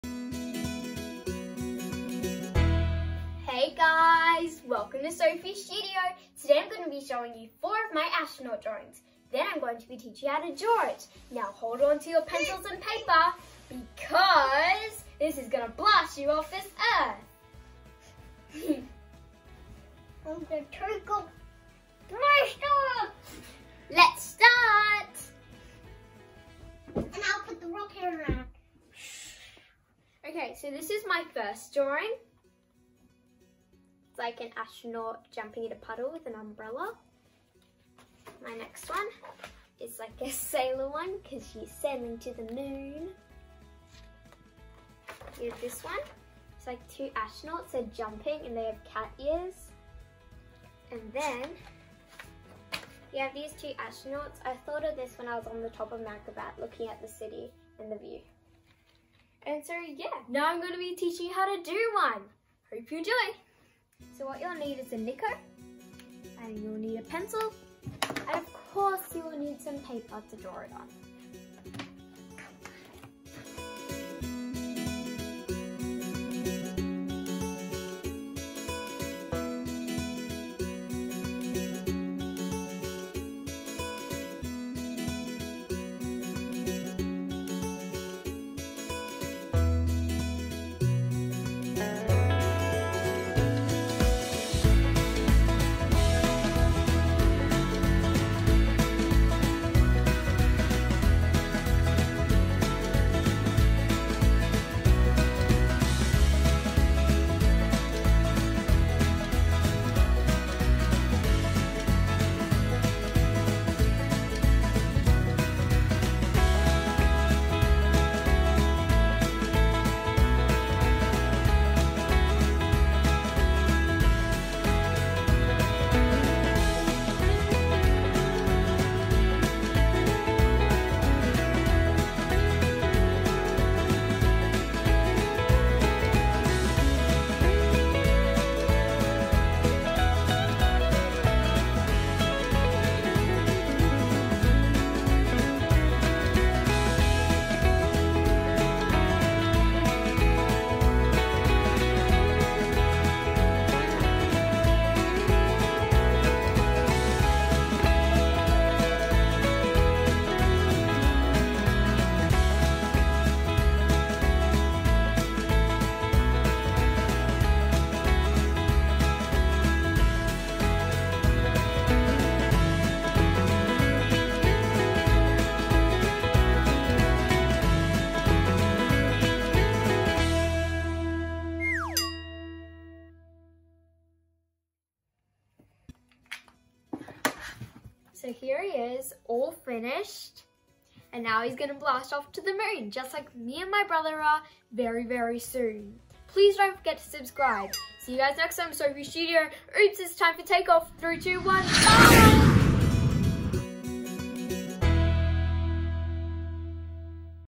Easy Astronaut Drawing
I love to draw!! I learnt this easy astronaut drawing and wanted to share with everyone. Drawing can be fun and easy!!! Have fun trying